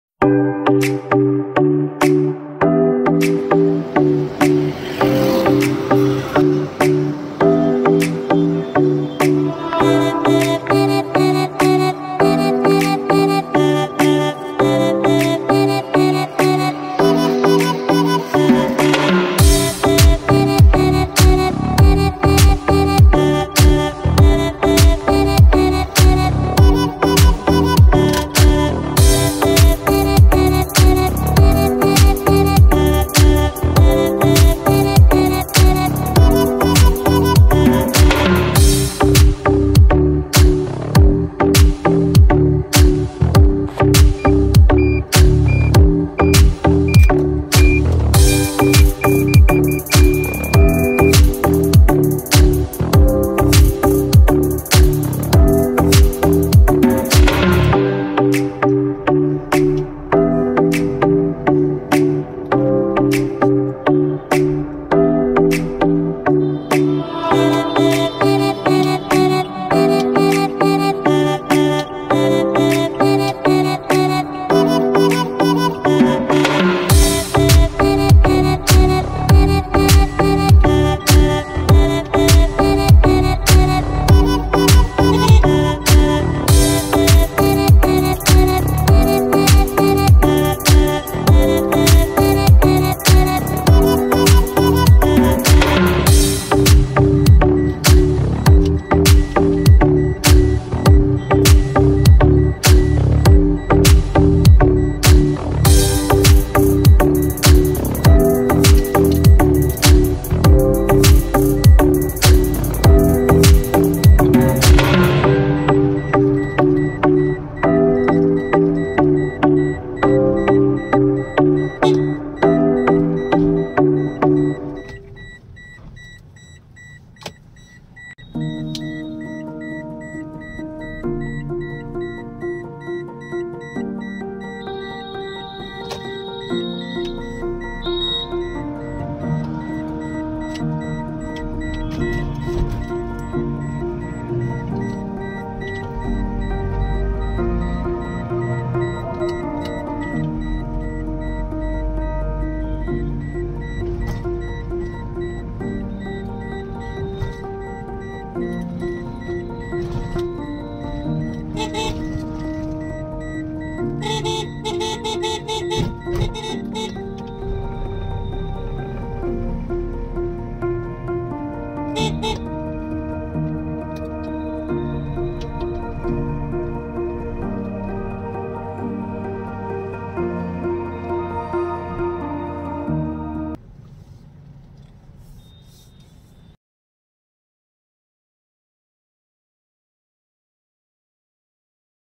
Night drive with Innova Crysta 2.4v 🚗🛣️. with features explained.
1. Push button start
2. Reverse camera
3. Back Sensor
4. Auto Headlamps(LED)